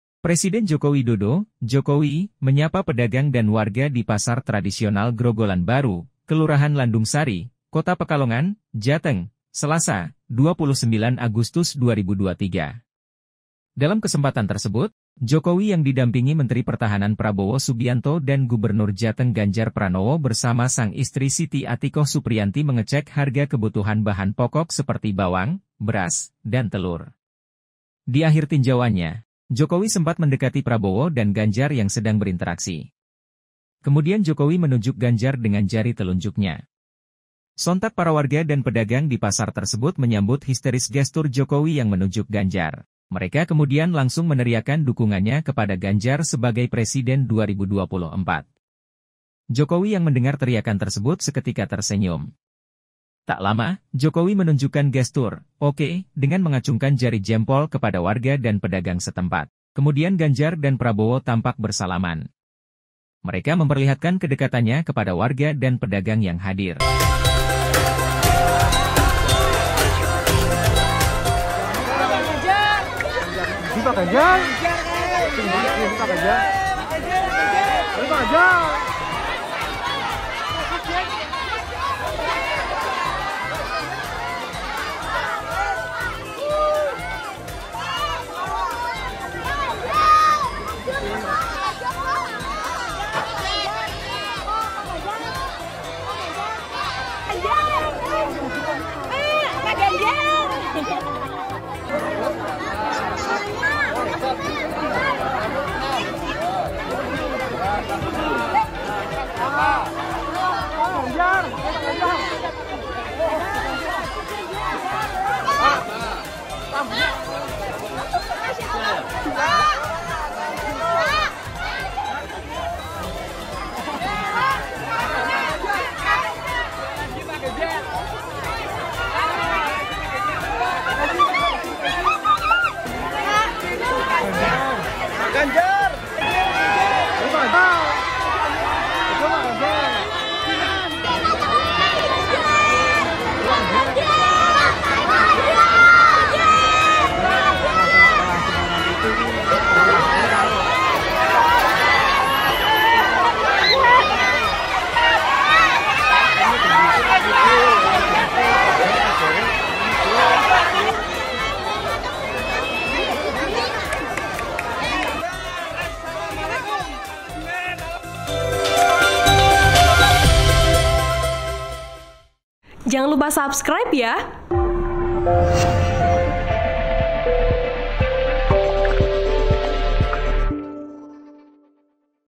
Kode Alam Jokowi Tunjuk dan Kasih Jempol, Warga Teriak Presiden
Presiden Joko Widodo (Jokowi) menyapa pedagang dan warga di Pasar Tradisional Grogolan Baru, Kelurahan Landungsari, Kota Pekalongan, Jateng, Selasa (29/8/2023).

Dalam kesempatan tersebut, Jokowi yang didampingi Menteri Pertahanan Prabowo Subianto dan Gubernur Jateng Ganjar Pranowo bersama sang istri Siti Atikoh Supriyanti mengecek harga kebutuhan bahan pokok seperti bawang, beras, dan telur.

Warga yang kedatangan para pejabat publik itu pun langsung antusias dan berebut salam satu per satu. Tak sedikit di antara mereka yang meminta foto selfie bersama ketiganya.

Di akhir tinjauannya, Jokowi sempat mendekati Prabowo dan Ganjar yang sedang berinteraksi. Kemudian Jokowi menujuk Ganjar dengan jari telunjuknya.